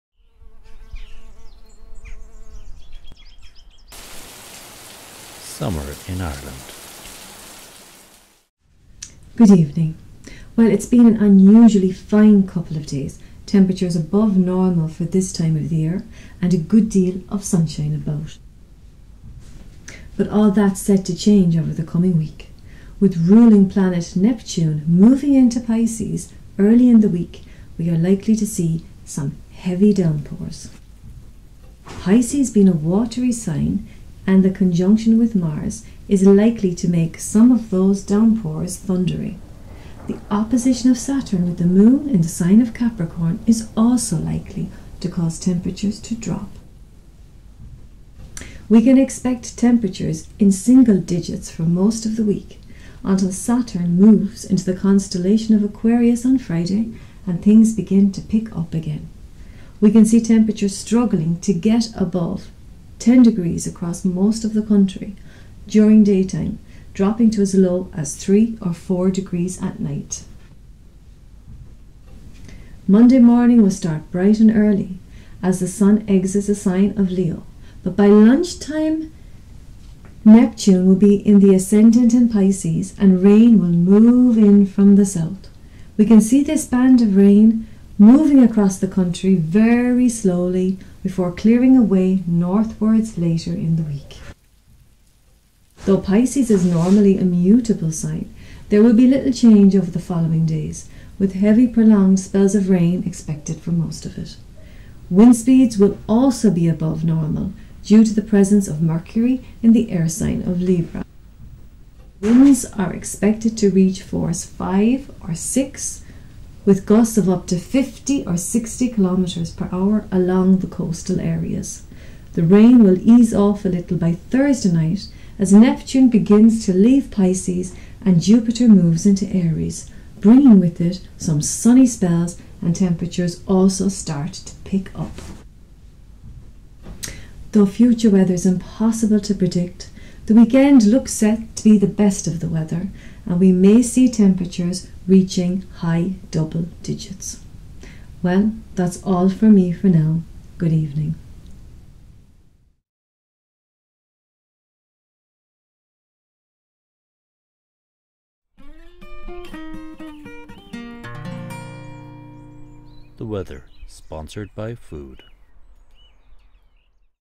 Irish Weather Forecast
Revealed at last. the precision science behind Irish Weather Forecasting!

Being a weather forecaster in Ireland is an unenviable task. With mental weather, four seasons in one day and everything covered with the catch-all phrase 'sunny spells and scattered showers' you may be wondering "how do they do it?"

Well, wonder no longer .